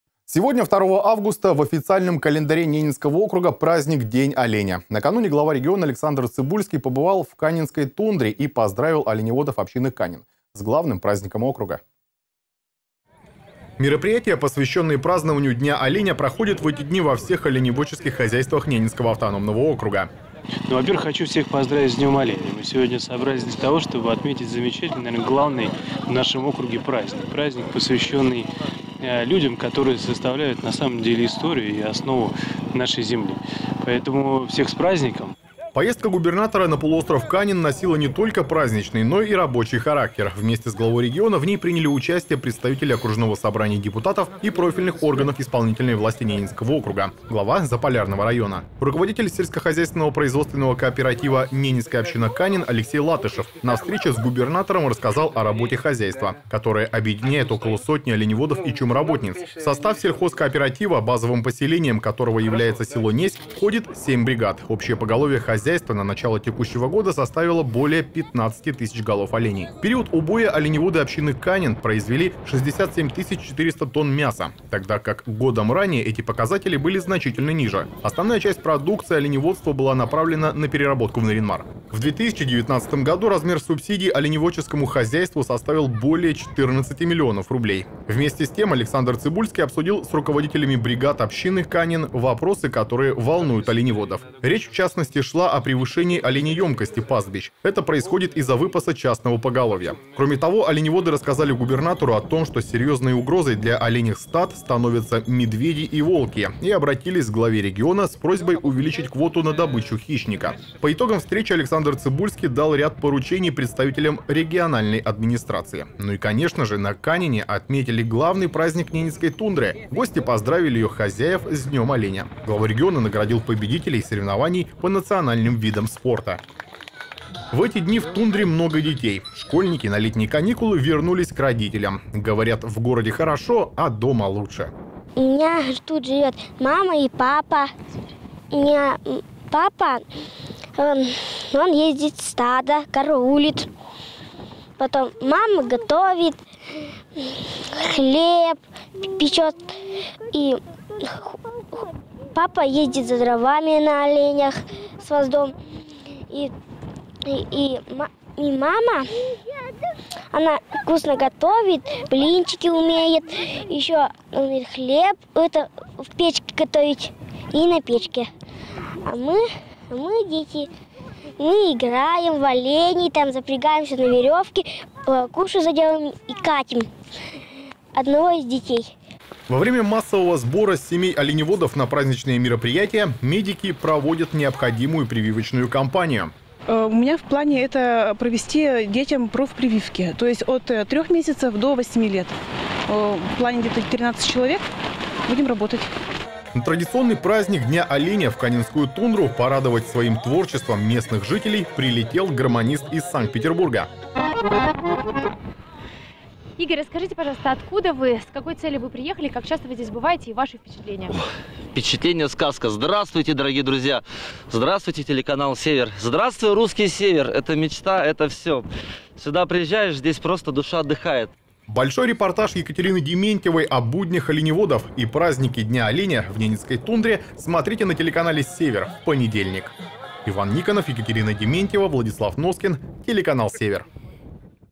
ДЕНЬ ОЛЕНЯ НА КАНИНЕ
Сегодня, 2 августа, в официальном календаре Ненецкого округа праздник - День оленя. Накануне глава региона Александр Цыбульский побывал в Канинской тундре и поздравил оленеводов общины «Канин» с главным праздником округа.